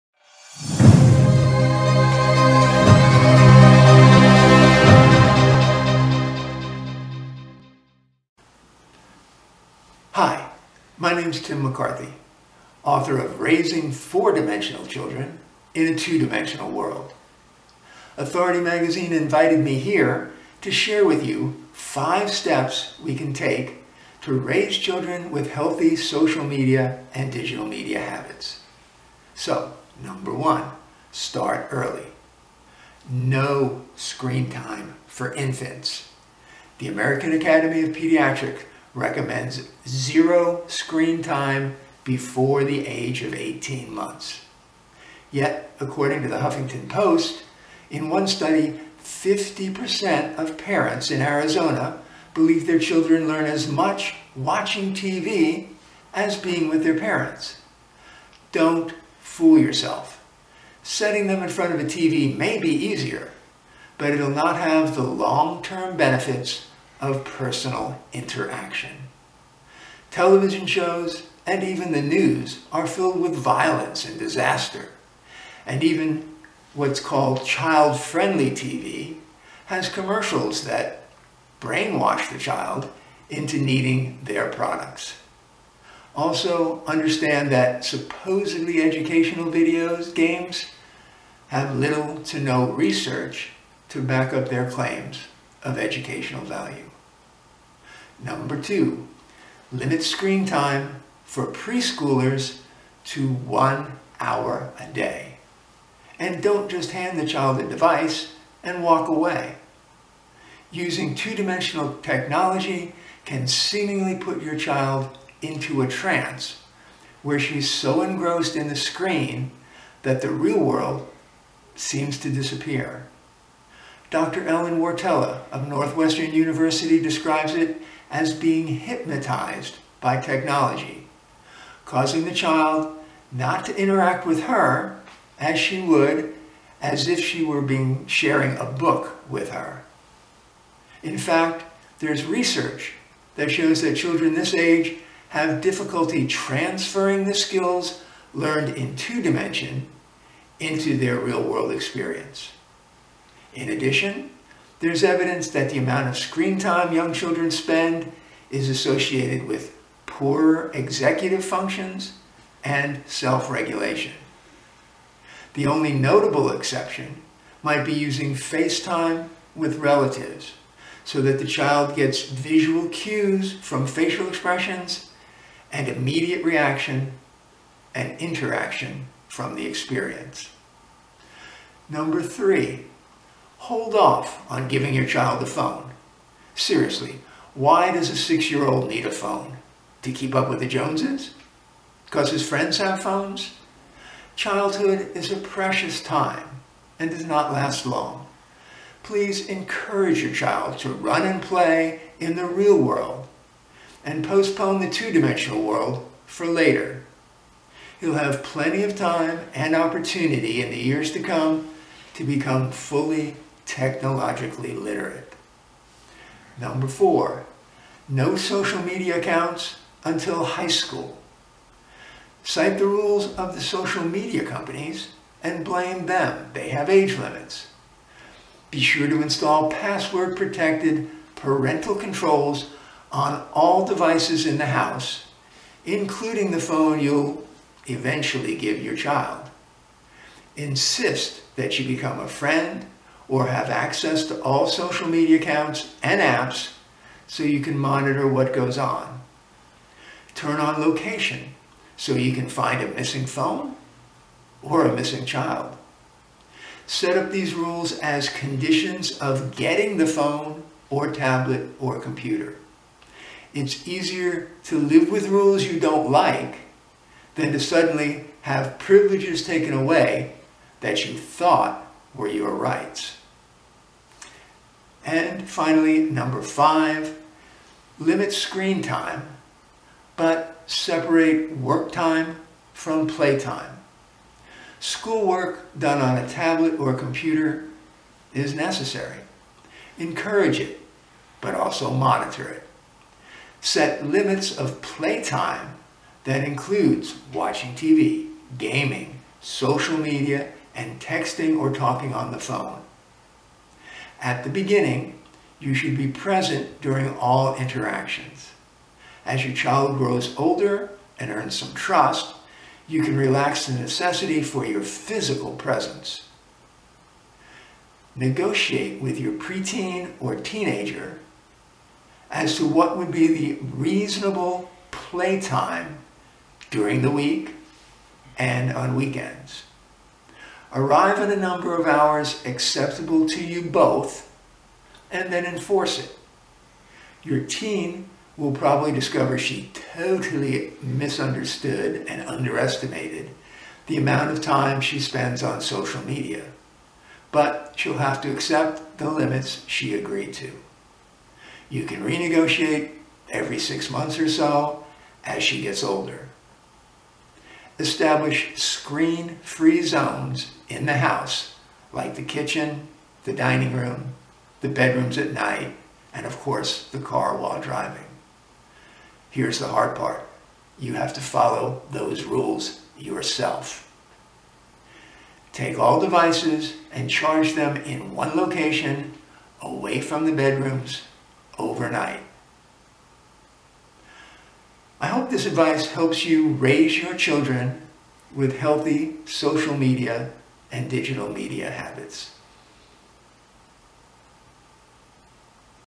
5 Things to Raise Children with Healthy Social and Digital Media Habits
Guide for parents to set healthy boundaries for their children on social media and other forms of digital media.